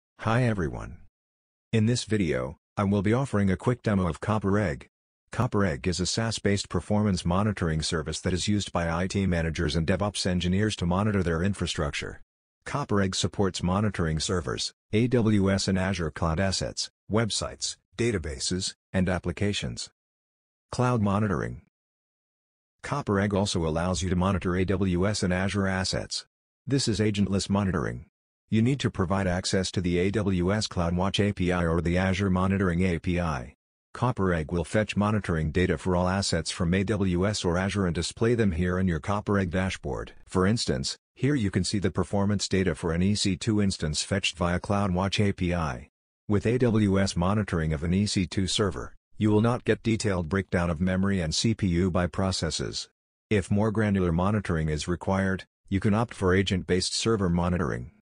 CopperEgg Demo Cloud Monitoring
In this video, I will be offering a quick demo of Cloud Monitoring using CopperEgg. CopperEgg is a SaaS based performance monitoring service that is used by IT Managers and DevOps Engineers to monitor their infrastructure. CopperEgg supports monitoring servers, AWS and Azure Cloud assets, Websites, Databases and Applications.